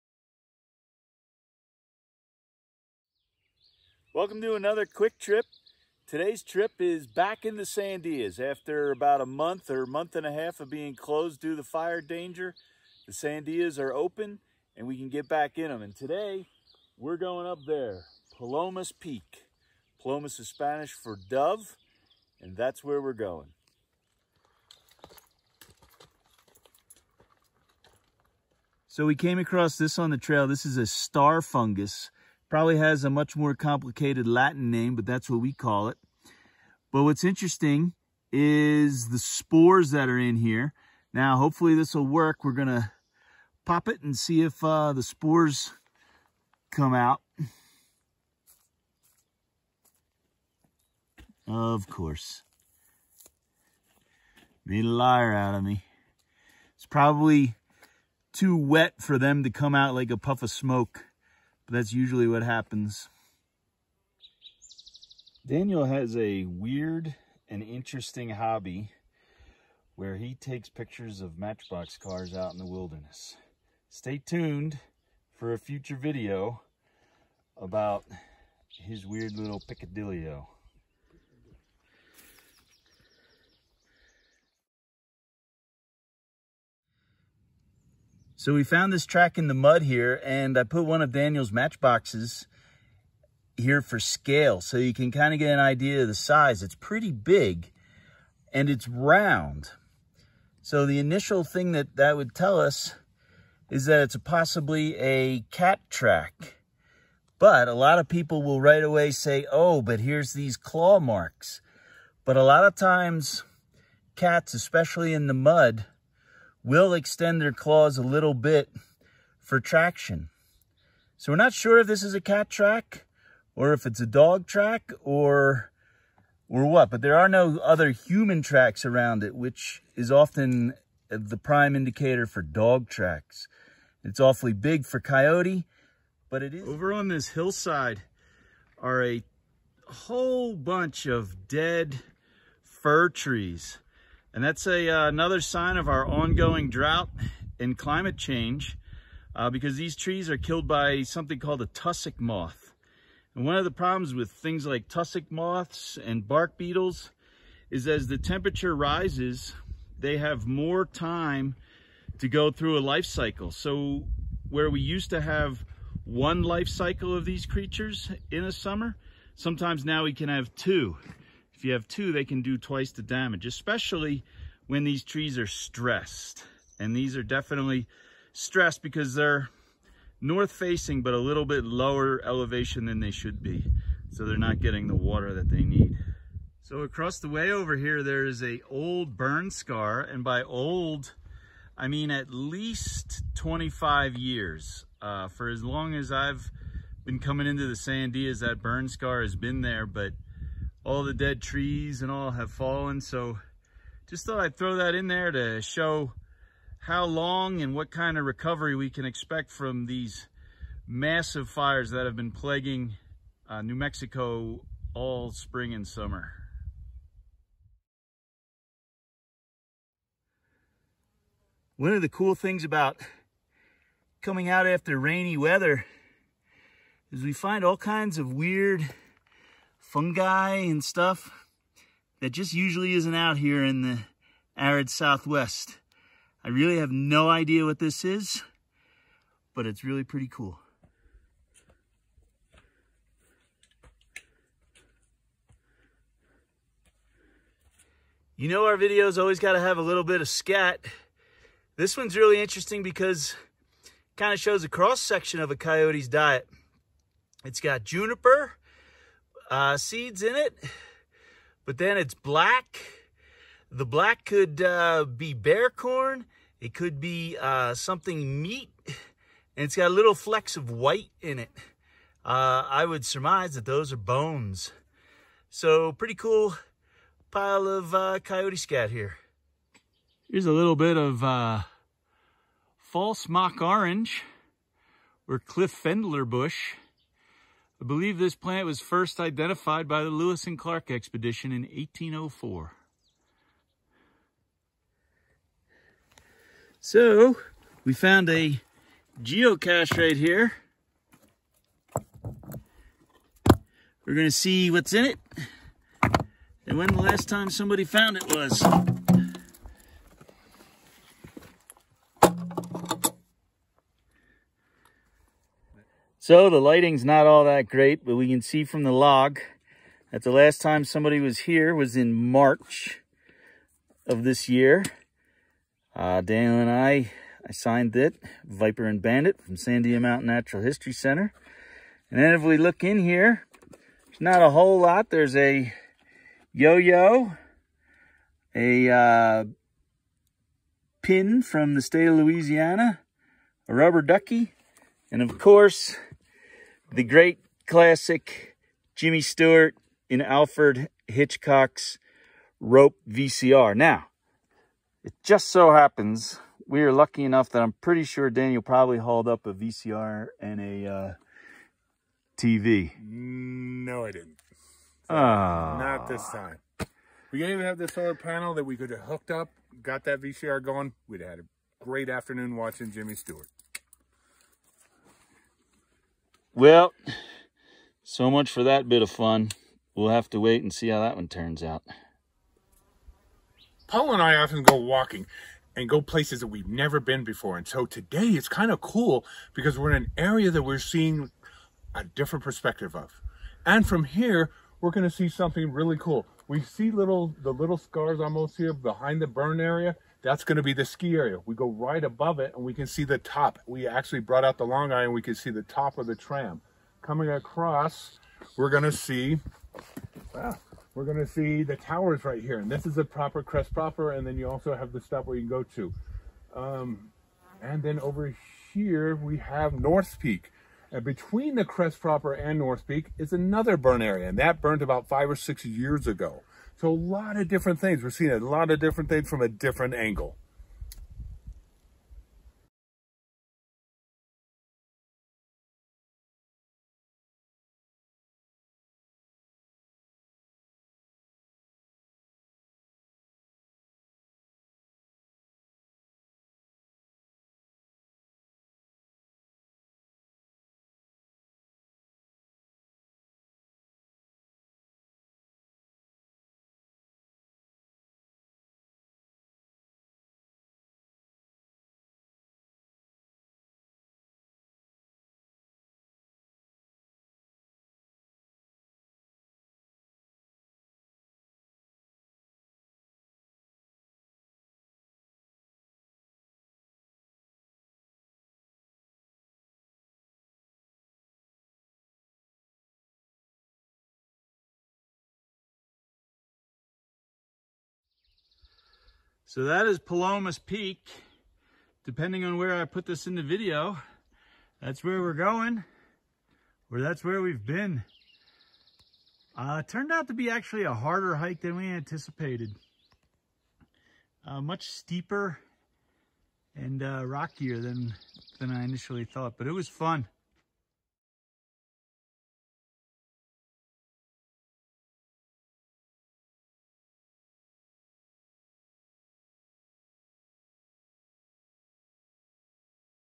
Palomas Peak, Sandia Mountains
Come along on a steep hike up to a lesser know peak in the Sandia Mountains during today's Quick Trip. See the sights, including puffballs, muddy tracks, burn scars, tussock moth damage, and more. Then soar above the peak with our drone footage!